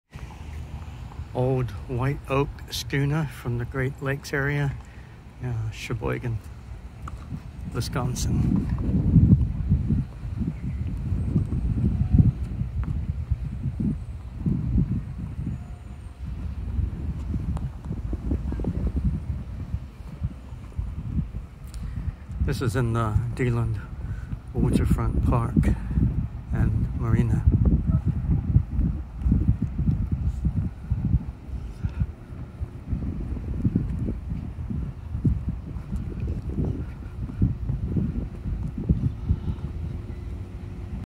Lottie Cooper Shipwreck at DELAND PARK WISCONSIN
A video of the Lottie Cooper shipwreck on display at Deland Park, Sheboygan, Wisconsin. I took the video 10 July 2020. The park is along the water front.